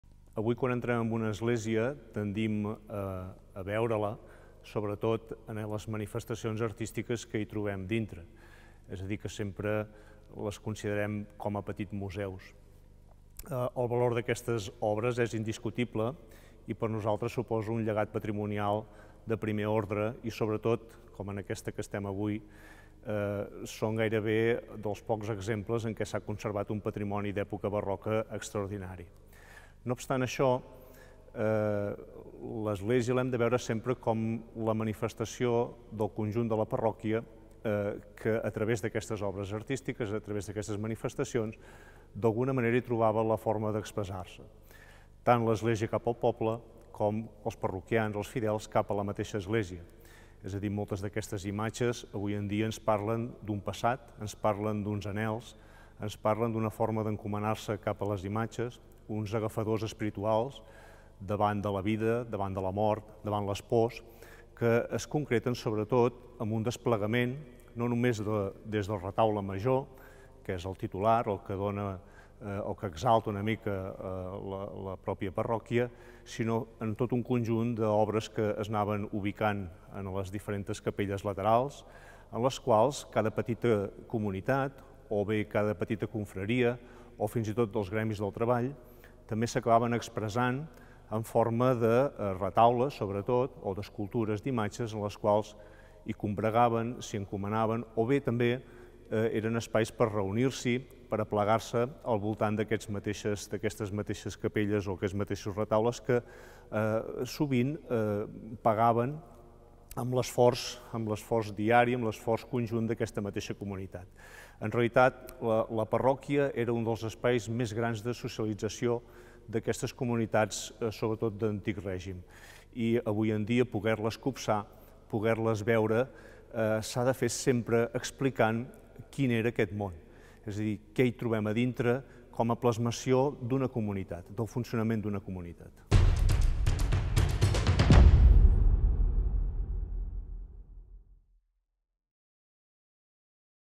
Catalonia Sacra | La importància de la sociabilitat parroquial
En aquest vídeo enregistrat a l'església de Sant Boi de Lluçanès, el Dr. Francesc Miralpeix (Universitat de Girona) sintetitza el paper vertebrador i socialitzador que han desenvolupat les parròquies arreu del territori i en destaca
🔴 Les esglésies s'han de veure com la manifestació del conjunt de la comunitat, que a través de les obres artístiques troba la forma d'expressar-se.
🔴 Les imatges ens parlen d'un passat, d'uns anhels, d'una forma de viure la fe, de posicionar-se davant de la vida, de la mort, de les pors...
🔴 La parròquia esdevé un dels espais més grans de socialització, més enllà de la seva funció religiosa.